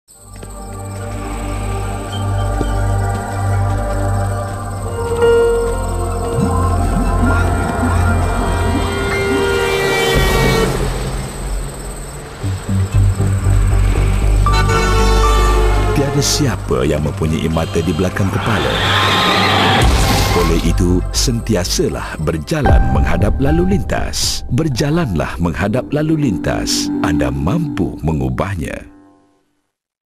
Iklan MOT Malaysia TVC 2006 (Pedestrian)
TV advertisement for Ministry of Transport Malaysia (in association with JKJR - Jabatan Keselamatan Jalan Raya), showcasing their commercial depicting awareness of pedestrian walking on the roadside in 2006.